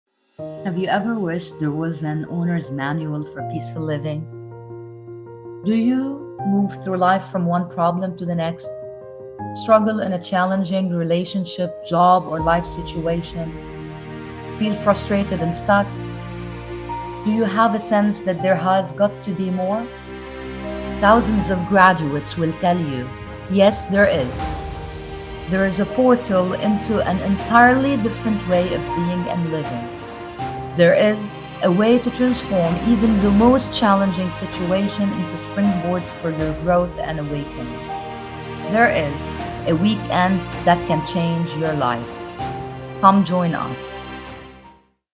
The Loyalty To Your Soul self limiting beliefs
If self-limiting beliefs, the fast pace life or emotional overwhelm hold you back, Loyalty to Your Soul offers an experiential solution.
Learn nine essential skills to shift perspectives, find balance, boost confidence, and improve communication.

Led in the MENA region by esteemed facilitators Dr. Maysar Sarieddine and Nour Fayad, LTYS provides an interactive learning environment where you’ll practice and embody these skills.

Join us in Beirut:
Date: May 10-11-12, 2024
Time: 9am-6:30pm
Location: Beirut Digital District

Join us in Dubai
Date: May 31- June 1 & 2, 2024
Time: 9am-6:30pm
Location: Keyani Wellness Center